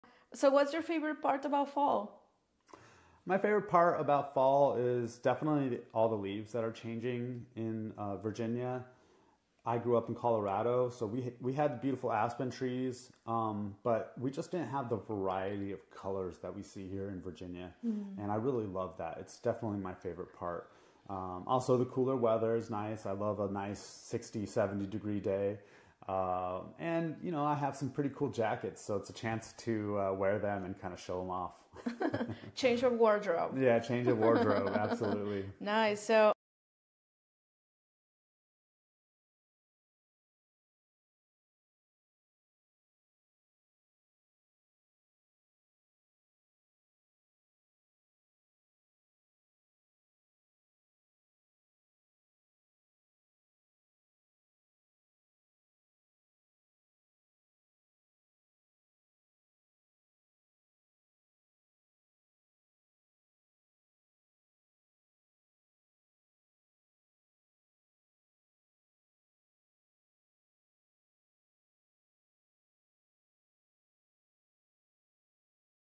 Fall Vibes: Embracing Change and Flavor🍂☕
In this video, we share our thoughts on the beloved pumpkin spice latte and the colorful transformation of fall in Virginia. From reduced sugar options to stylish jackets, join us as we celebrate the beauty of the season!